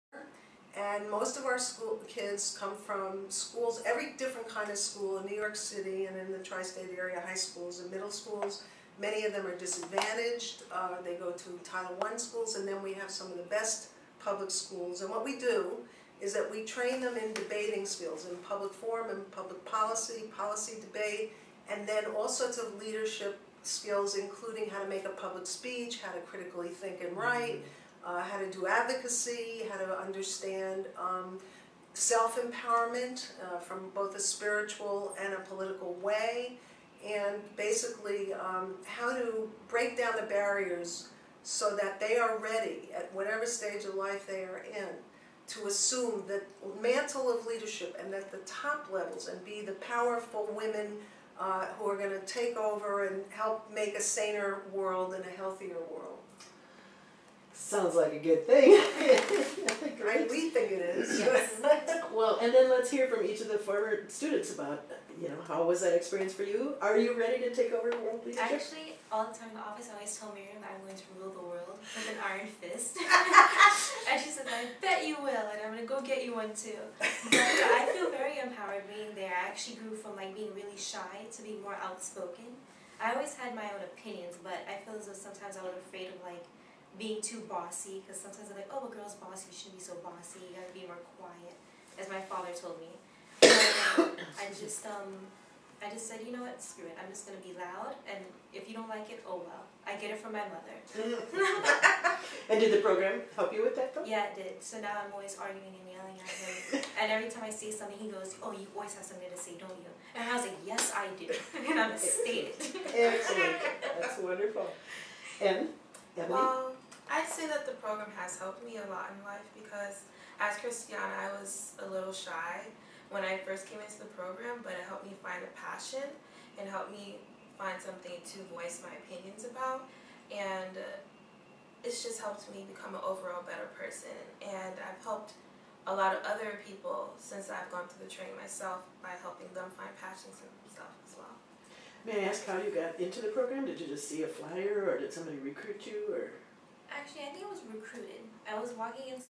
Manhattan Liz
"sample" of raw footage from interview with Liz Abzug and two graduates of the Bella Abzug Leadership Institute